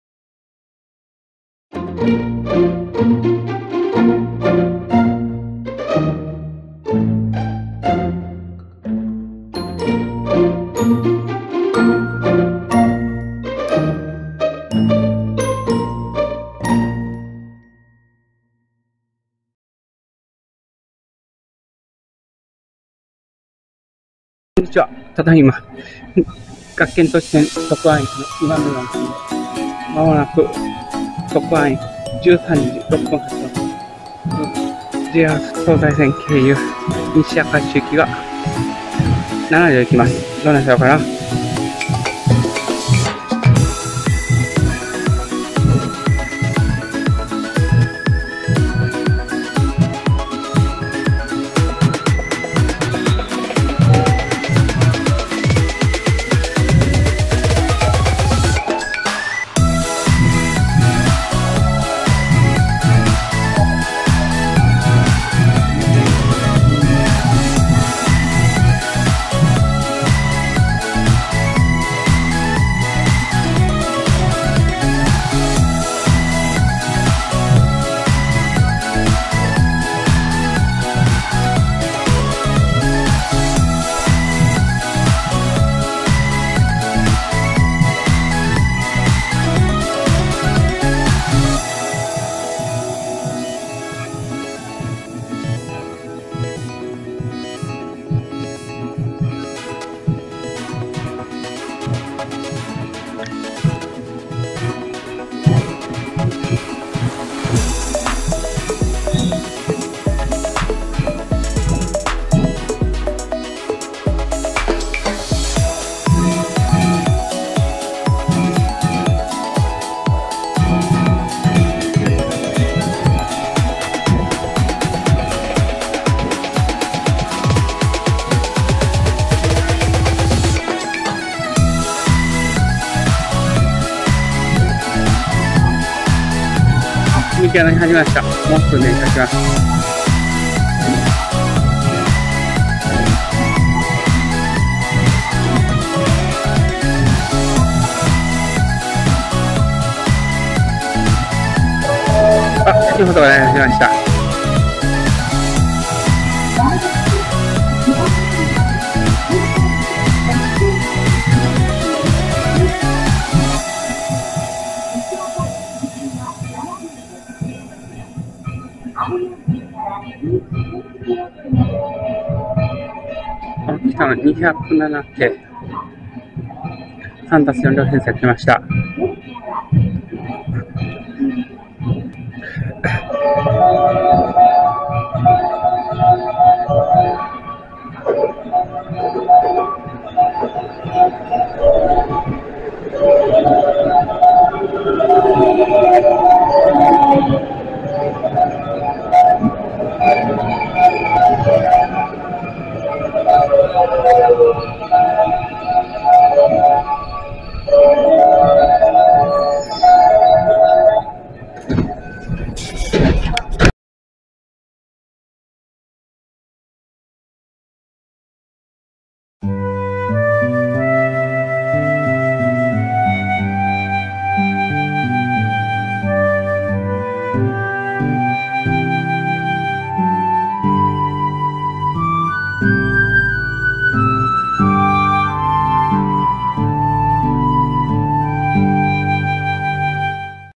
【鉄道ファン限定】学研都市線徳庵駅2番のりばに、207系3+4両編成の普通が入線
学研都市線徳庵駅2番のりばでの、207系3+4両編成の普通の入線シーンです。
徳庵駅13:06発の普通西明石行きです。